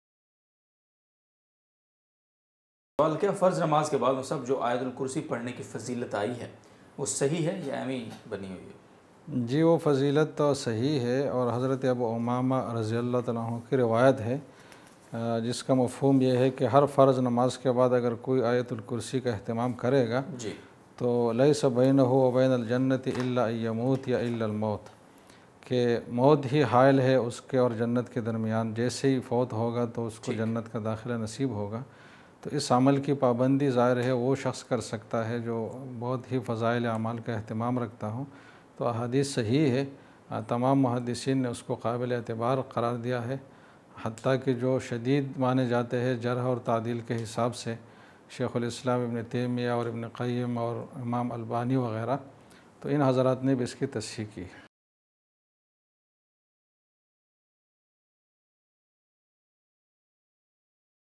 Farz Namaz Ky Bad Ayat-ul-Kursi Pharna-Mufti Husen Khalil Khel Sahib| Jamia-Tur-Rasheed Pakistan
Masail Short Clip # 1214
فرض نماز کے بعد آیت الکرسی پڑھنے کا حکم
مفتی محمد حسین خلیل خیل صاحب
استاذ جامعۃ الرشید کراچی